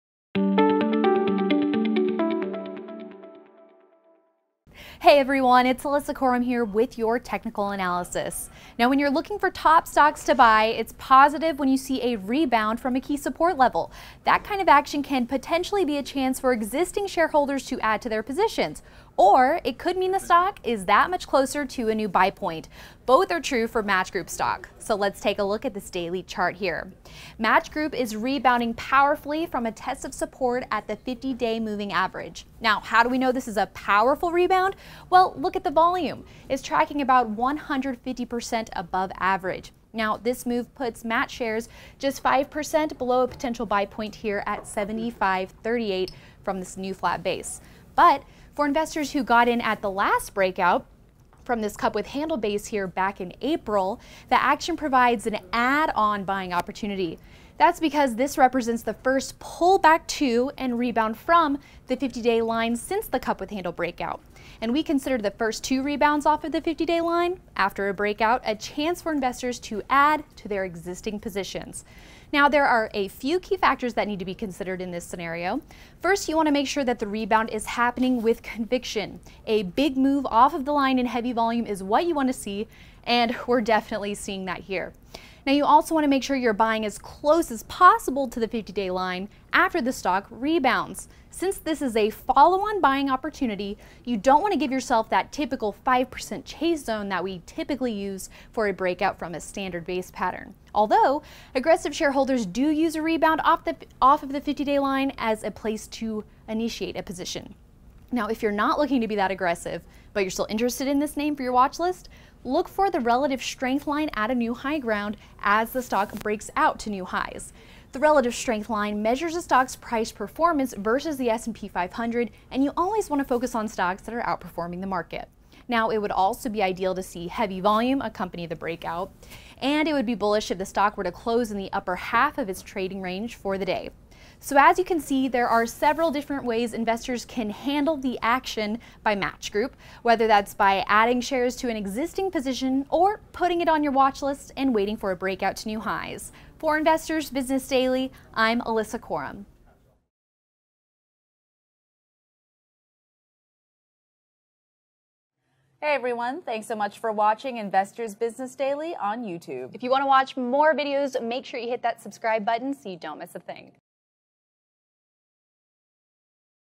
Technical Analysis: How To Add Shares When A Stock Rebounds Off The 50-Day Line
It’s positive when you see a rebound from a key support level — that kind of action can potentially be a chance for existing shareholders to add to their positions. Or it could mean the stock is that much closer to a new buy point. Both are true for Match Group stock.